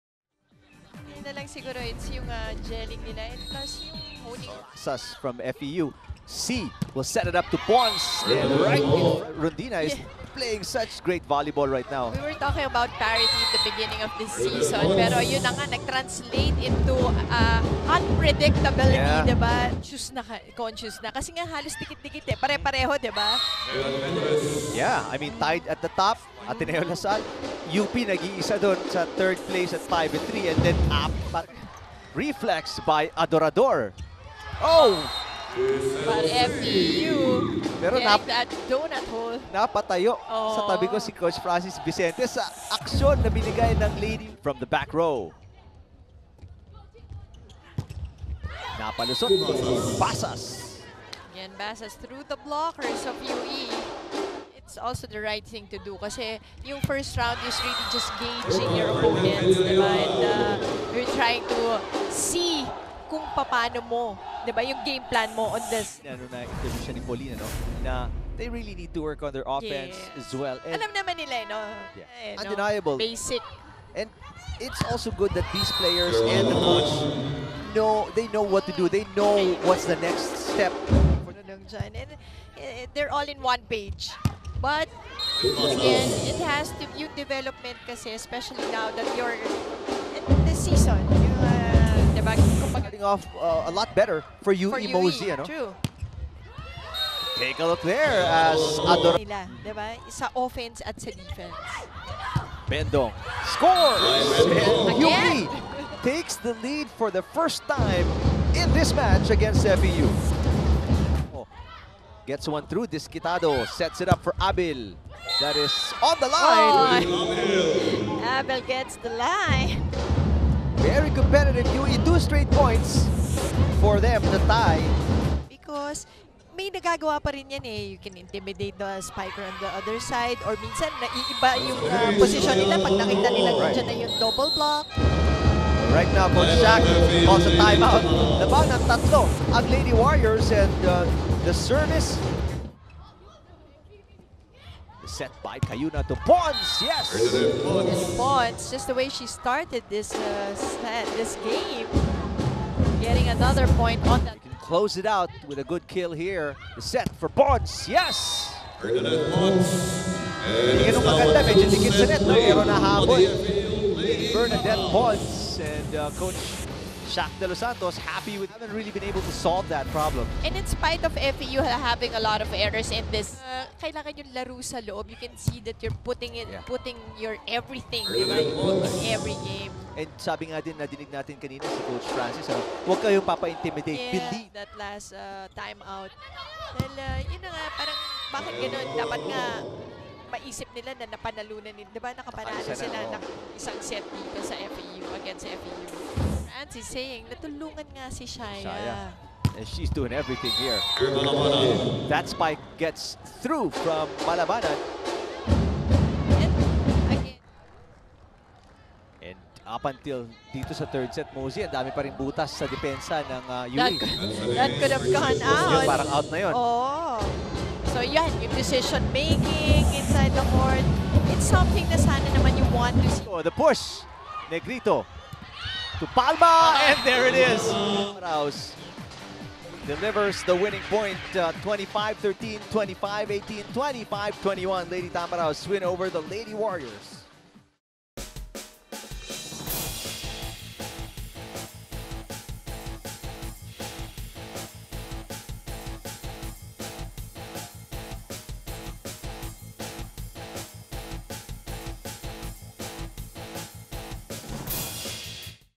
FEU vs. UE - March 10, 2016 | Game Highlights | UAAP 78 WV
UAAP Season 78 Women's Volleyball: Far Eastern University vs. University of the East | Game Highlights | March 10, 2016